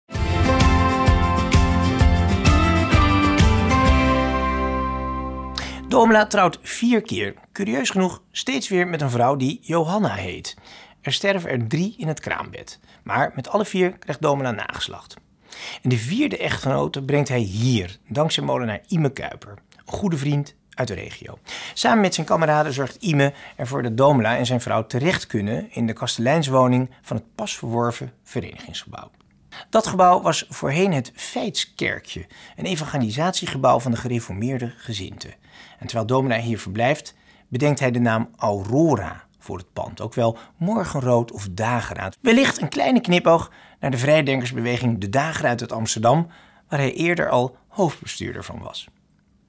Strijd voor gelijkheid - volksgebouw Aurora
Op deze plek stond voorheen het Volksgebouw Aurora, het verenigingsgebouw voor de socialistische beweging. Het werd in september 1919 gesloopt nadat het gekocht was door de Christelijke School. die de ruimte als doorgang naar de naastgelegen school zou gebruiken.

Het gebouw waar we tegenaan kijken is de voormalige Kruiskerk van de Protestantse gemeente Heerenveen. Deze werd gebouwd in 1921, twee jaar na de dood van Domela, en was tot 2012 in gebruik. Nu is dit gebouw een gemeentelijk monument en woonhuis voor ouderen.

Dankzij de hulp van zijn vriend Yme Kuiper kwam Domela hier terecht en kreeg het voormalige gebouw zijn naam. De hulp van Yme zou Domela nog vaker krijgen..